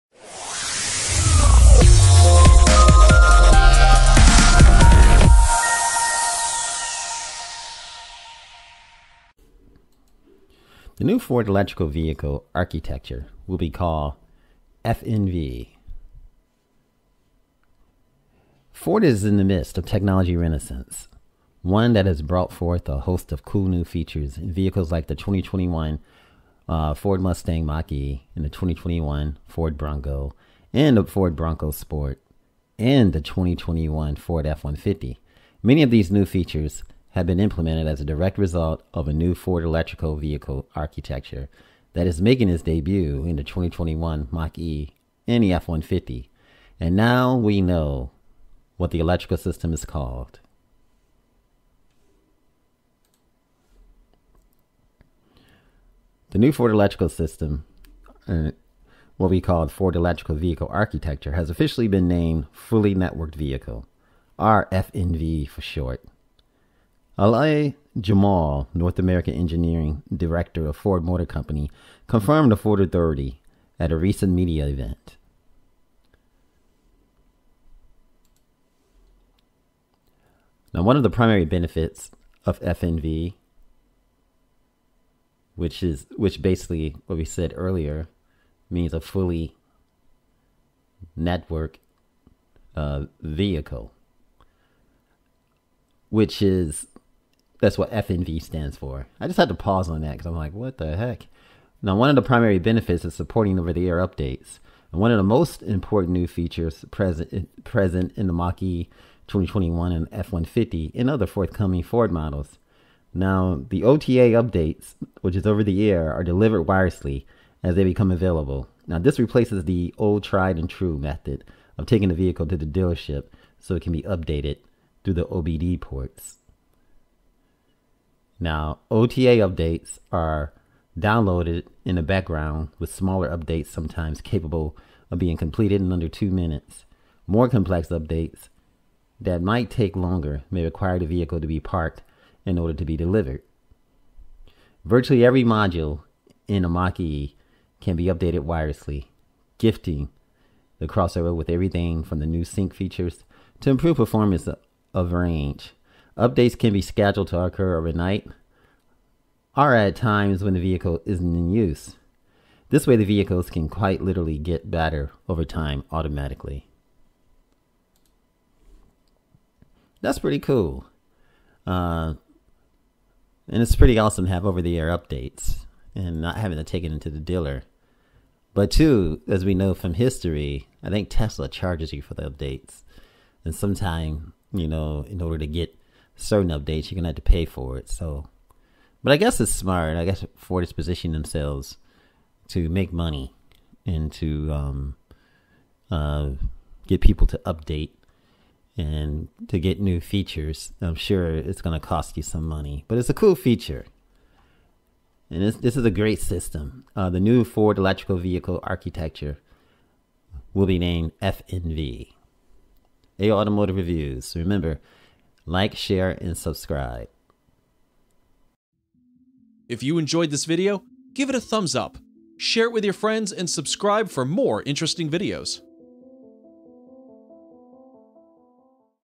New Ford Electrical Vehicle Architecture Will Be Named FNV
Ford is in the midst of a technology renaissance, one that has brought forth a host of cool new features in vehicles like the 2021 Ford Mustang Mach-E, 2021 Ford Bronco and Ford Bronco Sport, and the 2021 Ford F-150. Many of these new features have been implemented as a direct result of a new Ford electrical vehicle architecture that is making its debut in the 2021 Mach-E and F-150. And now, we know what that electrical system is called.